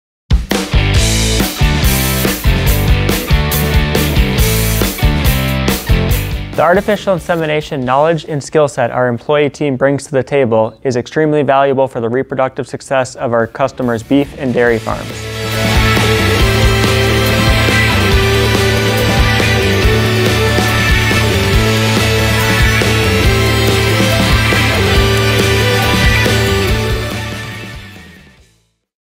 CentralStar's A.I. team brings value to our customers
CentralStar's Artificial Insemination (A.I) Specialists experience the rewarding career of helping dairy and beef customers excel in herd reproduction and productivity. Genetics and reproduction are key performance indicators on farms; so, our team's expertise in this area is valuable to our producers. Follow this link for details about our career opportunities AND to apply